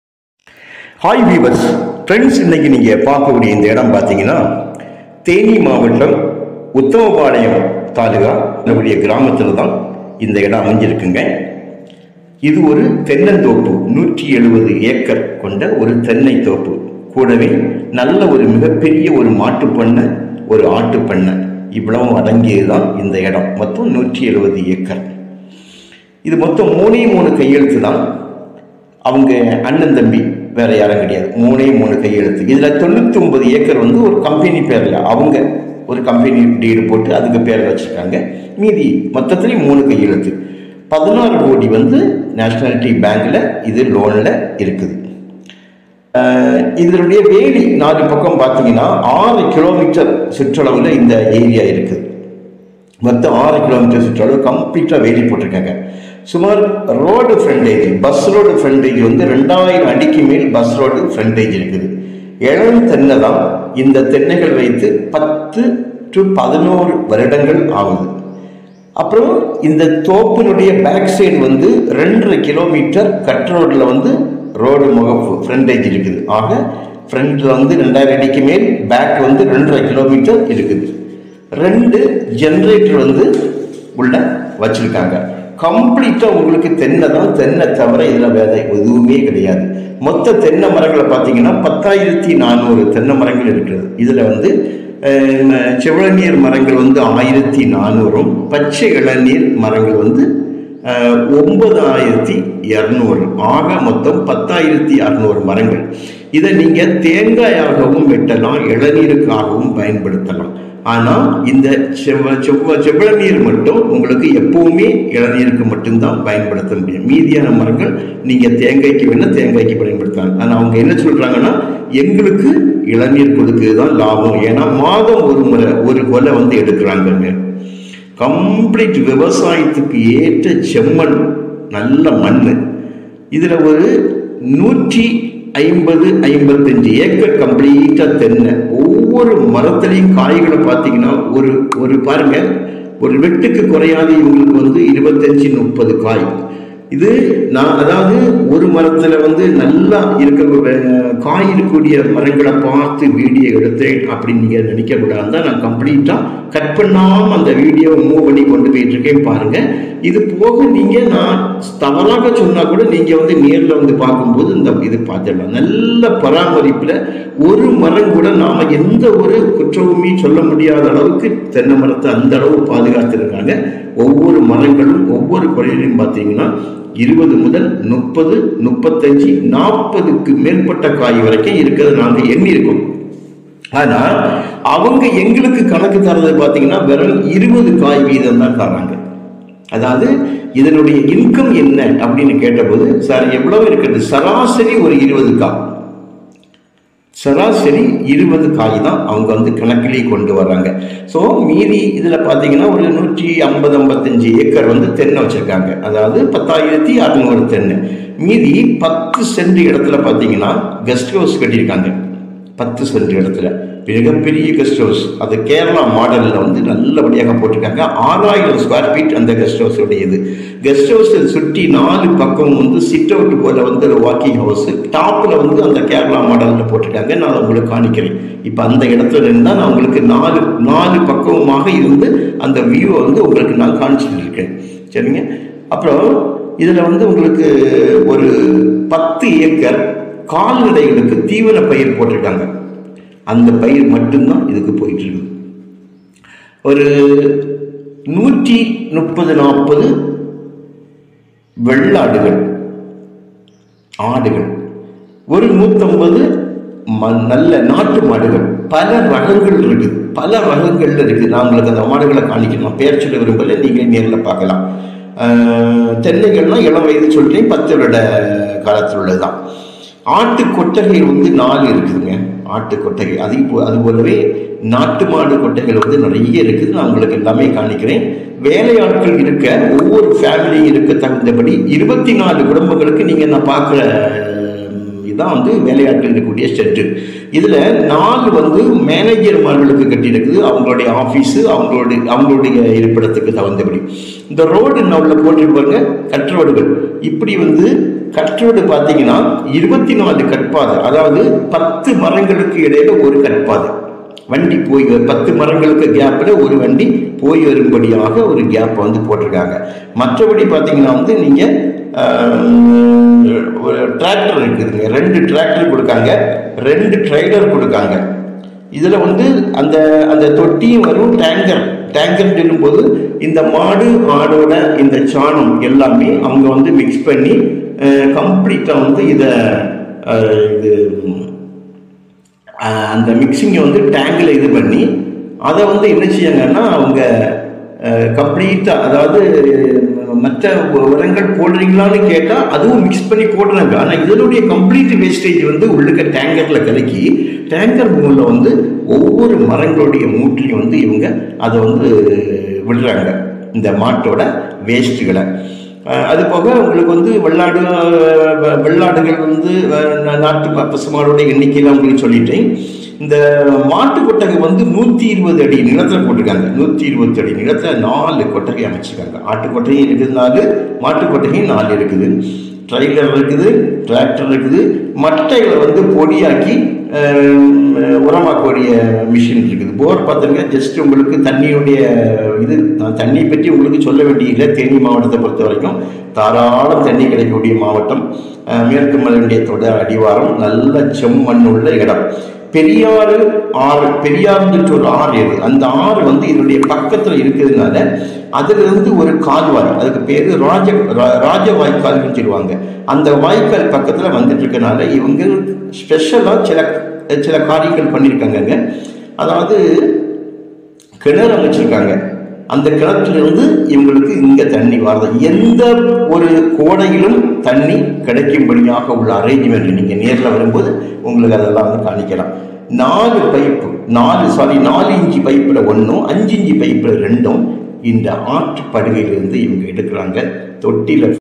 தேனி மாவட்டம் உத்தமபாளையம் தாலுகா அருகில் 168 ஏக்கர் தென்னந்தோப்பு நிலம் விற்பனைக்கு உள்ளது
தேனி மாவட்டம் உத்தமபாளையம் தாலுகா அருகில் 168 ஏகர் தென்னந்தோப்பு நிலம் விற்பனைக்கு உள்ளது ஒரு ஏக்கரை விலை 43 லட்சம் ஒரு சென்டின் விலை 43 ஆயிரம் மொத்தம் 10 ஆயிரத்து 600 தென்னை மரம் இருக்கு 1200 செவ்வாயினி மரம் இருக்கு 150 மாடு இருக்கு 150 ஆடு இருக்கு 16 கோடி ரூபாய் பேங்க் லோன்ல இருக்கு 99 ஏக்கர் கம்பெனி பெயரில் இருக்கு 150 மாடு 150 ஆடு இருக்கு 20 ஏக்கர்ல மாட்டுக்கு தீவனம் வளர்க்கறாங்க ரெண்டு கிணறு இருக்கு நான் 20 போர் இருக்கு ஒரு 6000 ஸ்கொயர் பீட்ல ஒரு வீடு இருக்கு 20 லேபர் கோட்ரஸ் இருக்கு மிக்க நன்றி மேலும் தொடர்புக்கு
ராஜா மகேந்திரன்